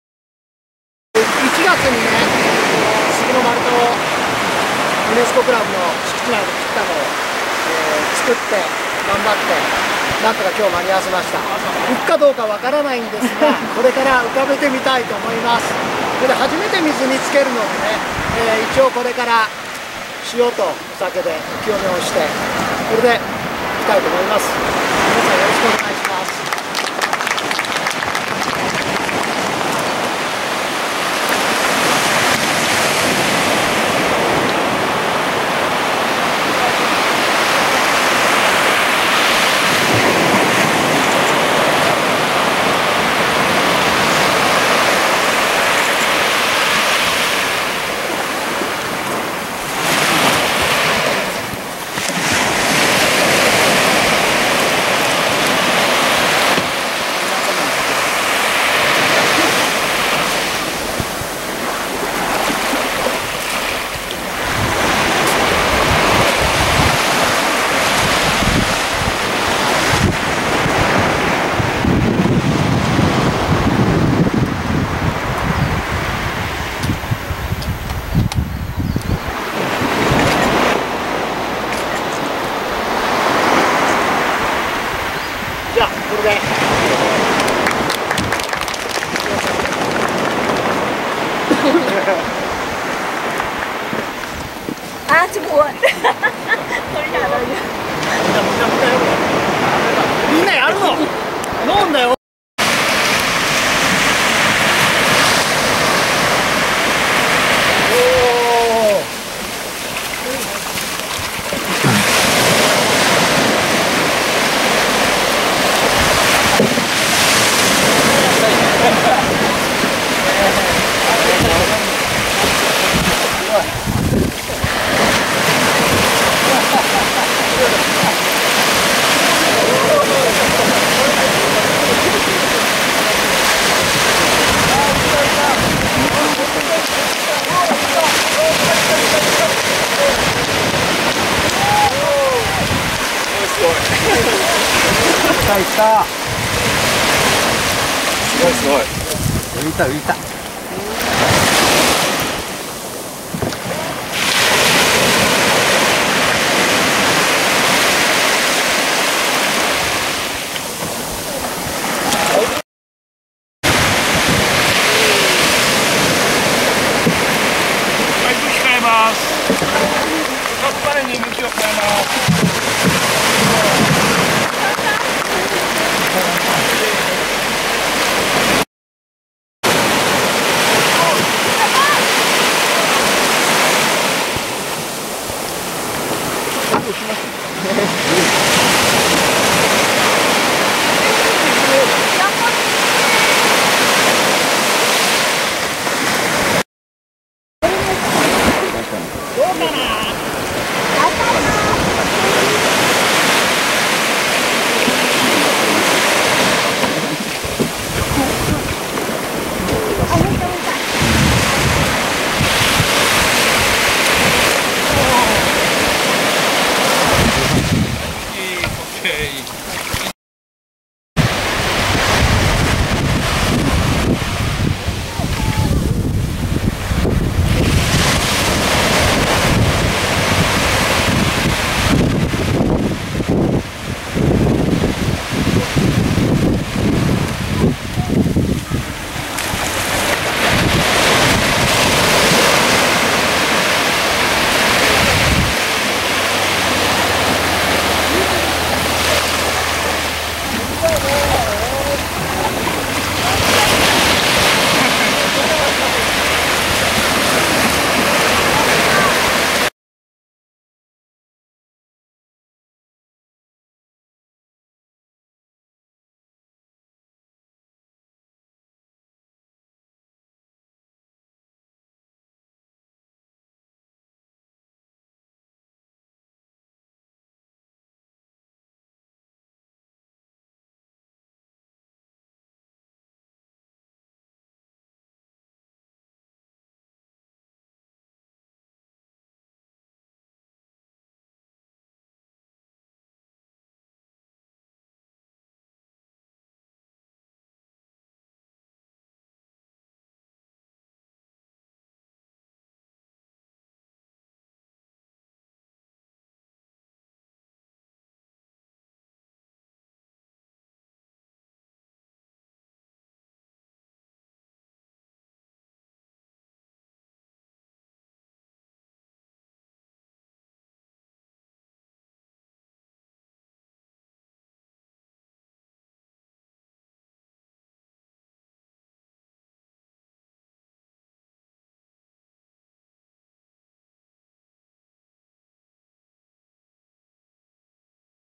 H281106カヌーすいすいプロジェクト
伊豆ユネスコクラブの「カヌーすいすいプロジェクト」
伊豆ユネスコクラブ主催で『南伊豆パドルスポーツ協会』協力で行われる事業
平成28年1月23日、40年過ぎた杉の伐採式しました。
切り出された杉は、パドル協会メンバーと子供たちの手で、ゲレンデ内の『陸の造船所』に於いて６か月間掛けて、くり抜かれてカヌーの形になってきました。
10月8日には色も塗られ、今日の進水式となりました。
カヌーに使われなった杉の残りは、安東幹事指導のもと、子供たちのデザインとペイントでトーテムポールにと作り上げられ、４０年生きてきた杉の命を無駄なく活用させていただく事業です。

今日は無事海に浮かび、思ったより安定感がある船体に関係者一同大喜びでした。